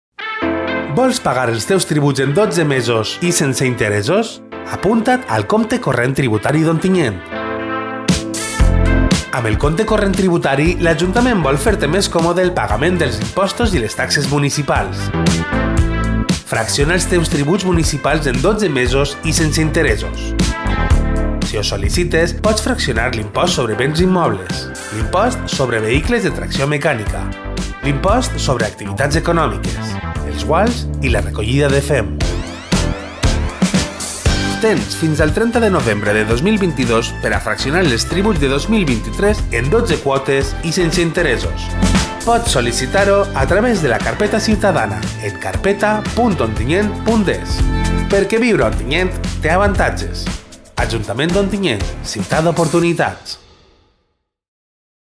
Compte Corrent Tributari 2022
Vols pagar els teus tributs municipals en 12 mesos i sense interessos? Apunta't al Compte Corrent Tributari d'Ontinyent!

✅ Pots fraccionar el rebut del fem, l'IAE, l'IBI, l'IVTM i els guals.
📆 Tens fins al 30 de novembre de 2022
📌 Entra en carpeta.ontinyent.es i apunta't!